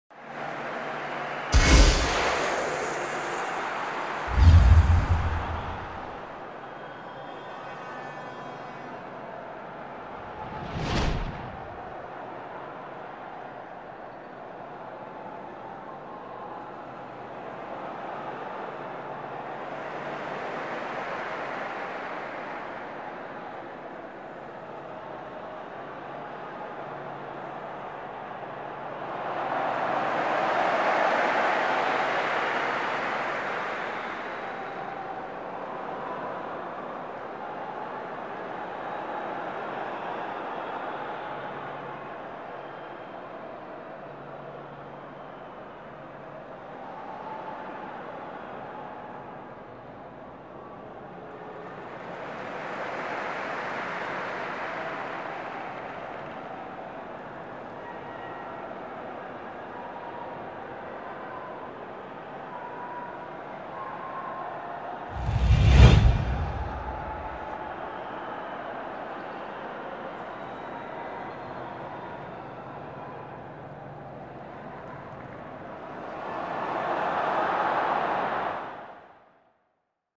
Football: Passes and coordination
Good exercise to improve the precision of the passes.